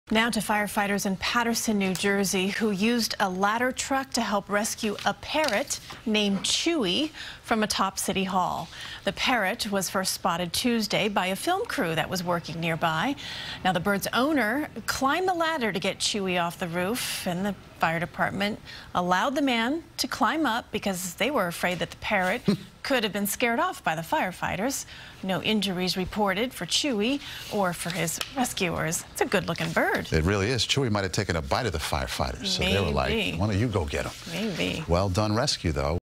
Escaped Parrot Rescued From Roof At Paterson City Hall
Firefighters in Paterson, New Jersey, used a ladder truck to help rescue a parrot named Chewy from atop City Hall.